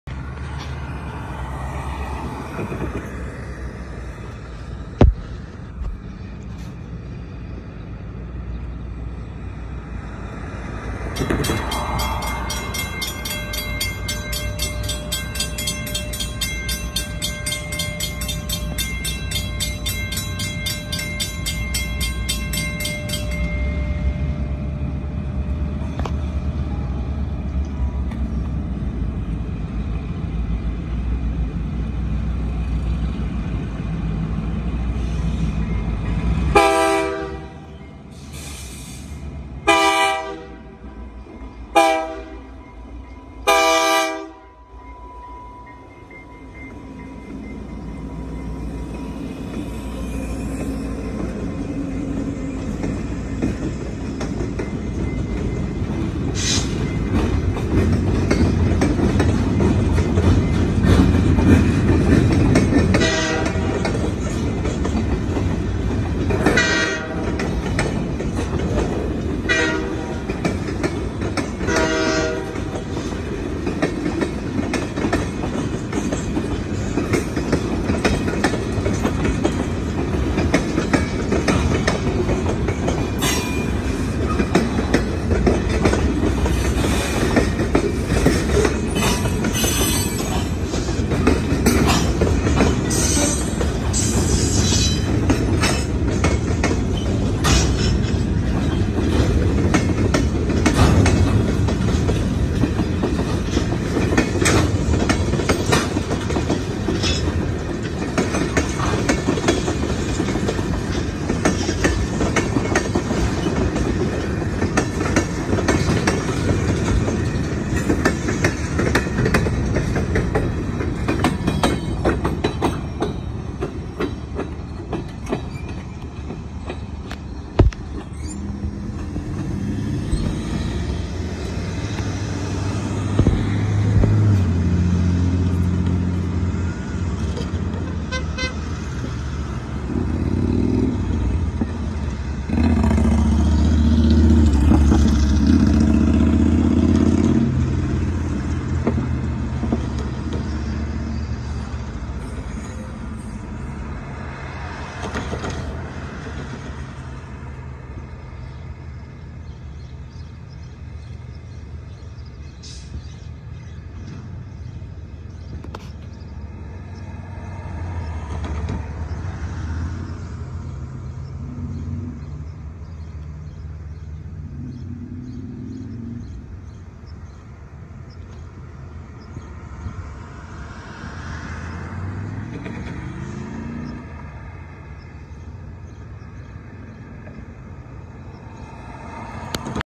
Gs2
FGLKs Railway Gs2 going across Seymour street in Auburn,NY ( taken 5/22/20)